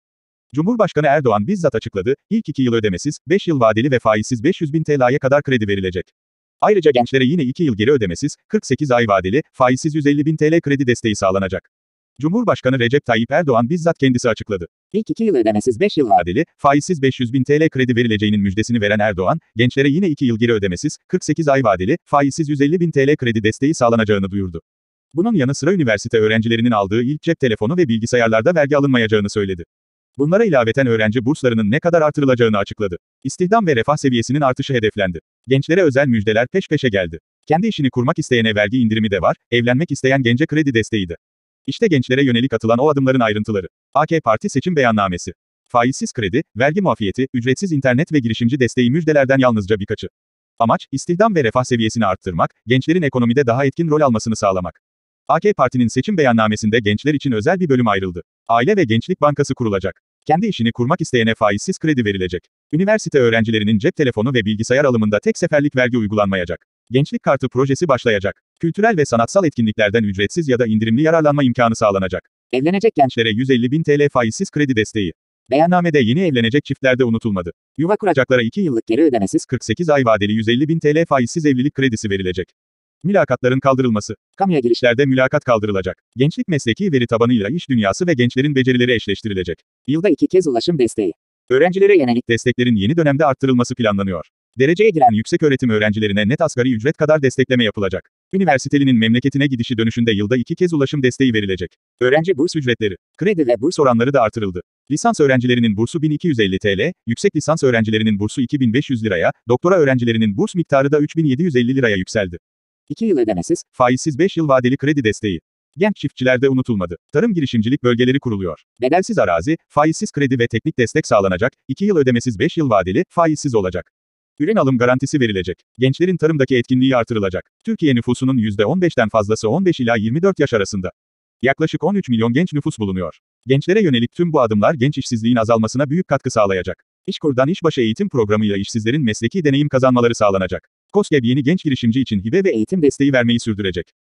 İlk 2 yıl ödemesiz 5 yıl vadeli, faizsiz 500 bin TL kredi verilecek
Cumhurbaşkanı Erdoğan bizzat açıkladı: ilk 2 yıl ödemesiz, 5 yıl vadeli ve faizsiz 500 bin TL’ye kadar kredi verilecek! Ayrıca gençlere yine 2 yıl geri ödemesiz, 48 ay vadeli, faizsiz 150 bin TL kredi desteği sağlanacak.

Cumhurbaşkanı Recep Tayyip Erdoğan bizzat kendisi açıkladı. İlk 2 yıl ödemesiz 5 yıl vadeli, faizsiz 500 bin TL kredi verileceğinin müjdesini veren Erdoğan, gençlere yine 2 yıl geri ödemesiz, 48 ay vadeli, faizsiz 150 bin TL kredi desteği sağlanacağını duyurdu. Bunun yanı sıra üniversite öğrencilerinin aldığı ilk cep telefonu ve bilgisayarlarda vergi alınmayacağını söyledi. Bunlara ilaveten öğrenci burslarının ne kadar artırılacağını açıkladı.

İstihdam ve refah seviyesinin artışı hedeflendi. Gençlere özel müjdeler peş peşe geldi. Kendi işini kurmak isteyene vergi indirimi de var, evlenmek isteyen gence kredi desteği de. İşte gençlere yönelik atılan o adımların ayrıntıları.

AK Parti Seçim Beyannamesi
Faizsiz kredi, vergi muafiyeti, ücretsiz internet ve girişimci desteği müjdelerden yalnızca birkaçı. Amaç; istihdam ve refah seviyesini arttırmak, gençlerin ekonomide daha etkin rol almasını sağlamak. AK Parti'nin seçim beyannamesinde gençler için özel bir bölüm ayrıldı. Aile ve Gençlik Bankası kurulacak. Kendi işini kurmak isteyene faizsiz kredi verilecek. Üniversite öğrencilerinin cep telefonu ve bilgisayar alımında tek seferlik vergi uygulanmayacak.

Gençlik kartı projesi başlayacak
Kültürel ve sanatsal etkinliklerden ücretsiz ya da indirimli yararlanma imkanı sağlanacak.

Evlenecek Gençlere 150 Bin TL Faizsiz Kredi Desteği

Beyannamede yeni evlenecek çiftlerde unutulmadı. Yuva kuracaklara 2 yıllık geri ödemesiz 48 ay vadeli 150.000 TL faizsiz evlilik kredisi verilecek.

Mülakatların Kaldırılması
Kamuya girişlerde mülakat kaldırılacak. Gençlik mesleki veri tabanıyla iş dünyası ve gençlerin becerileri eşleştirilecek.

Yılda 2 Kez Ulaşım Desteği
Öğrencilere yönelik desteklerin yeni dönemde arttırılması planlanıyor. Dereceye giren yükseköğretim öğrencilerine net asgari ücret kadar destekleme yapılacak. Üniversitelinin memleketine gidişi dönüşünde yılda 2 kez ulaşım desteği verilecek.

Öğrenci Burs Ücretleri
Kredi ve burs oranları da artırıldı. Lisans öğrencilerinin bursu 1.250 TL, yüksek lisans öğrencilerinin bursu 2.500 liraya, doktora öğrencilerinin burs miktarı da 3.750 liraya yükseldi.

2 Yıl Ödemesiz, faizsiz 5 yıl Vadeli Kredi Desteği
Genç çiftçiler de unutulmadı. Tarım girişimcilik bölgeleri kuruluyor. Bedelsiz arazi, faizsiz kredi ve teknik destek sağlanacak. 2 yıl ödemesiz 5 yıl vadeli, faizsiz olacak. Ürün alım garantisi verilecek. Gençlerin tarımdaki etkinliği artırılacak.

Türkiye nüfusunun %15’den fazlası 15-24 yaş arasında. Yaklaşık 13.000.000 genç nüfus bulunuyor. Gençlere yönelik tüm bu adımlar genç işsizliğin azalmasına büyük katkı sağlayacak.

İŞKUR'dan iş başı eğitim programıyla işsizlerin mesleki deneyim kazanmaları sağlanacak. KOSGEB yeni genç girişimci için hibe ve eğitim desteği vermeyi sürdürecek.